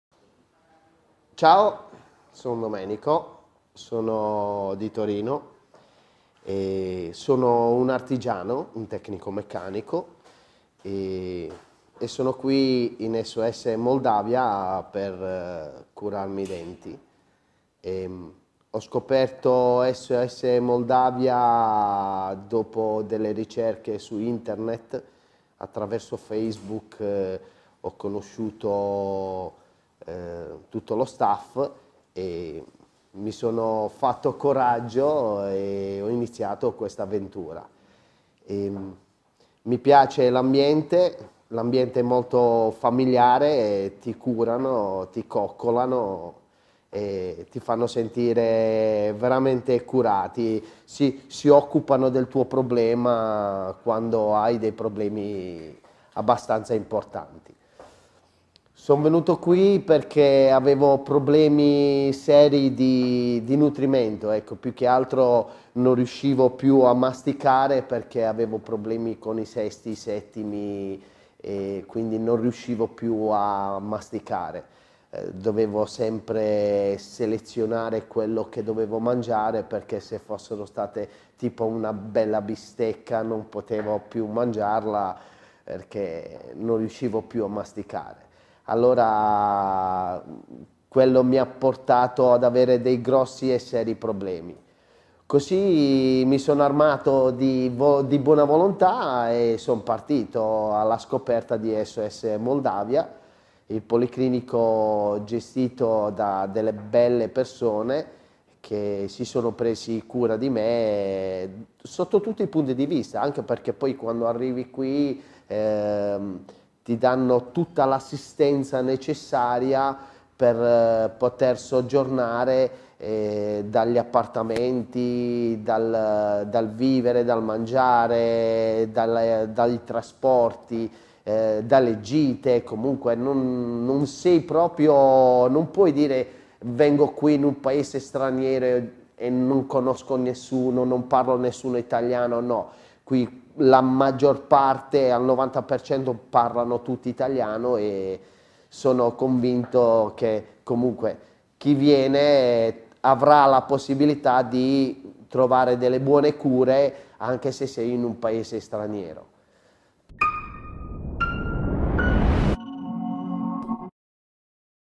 Dr Palmas: Caso risolto con impianti dentali, la testimonianza di Domenico
Domenico si è affidato ad SOS Turismo Dentale, il policlinico del Dr. Palmas, per sistemare la sua bocca afflitta da problemi dentali, con una soluzione all on 6 .

" Sono tornato a sorridere liberamente e a non dovermi coprire la bocca già con i denti provvisori"
afferma Domenico già dopo la prima parte di implantologia.

Dopo varie ricerche internet, nell'arco di oltre 3-4 mesi si è convinto a contattare SOS Turismo Dentale.

In questo video racconta la sua esperienza con i membri del team e con il Dr. Palmas, che senza dubbio consiglierebbe a chiunque ha problemi dello stesso tipo (bocche da riabilitare completamente, all-on-4, all-on-6, all-on-8).